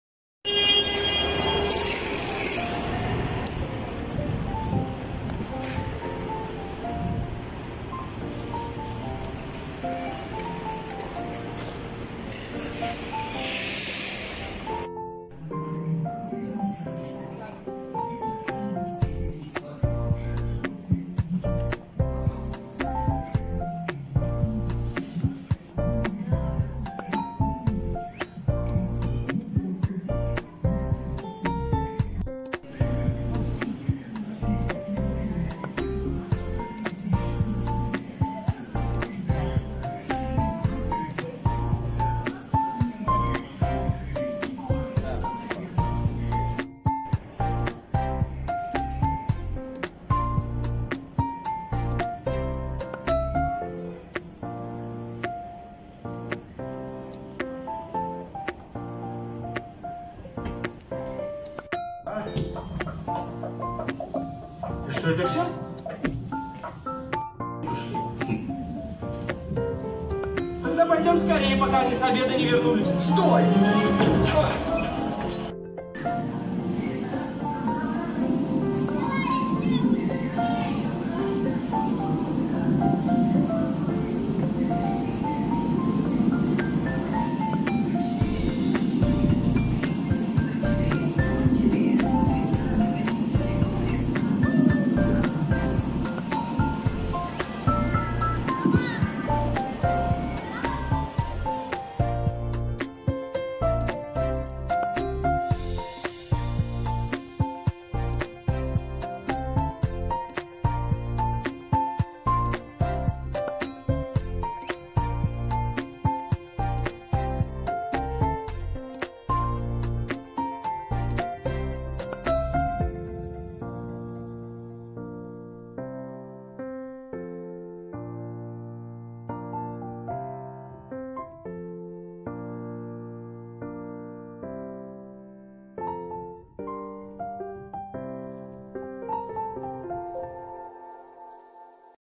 Наш семейный день вместе 😍
Отлично провели день с семьей. Покатались на коньках, прогулялись по магазинам и даже сходили в кино.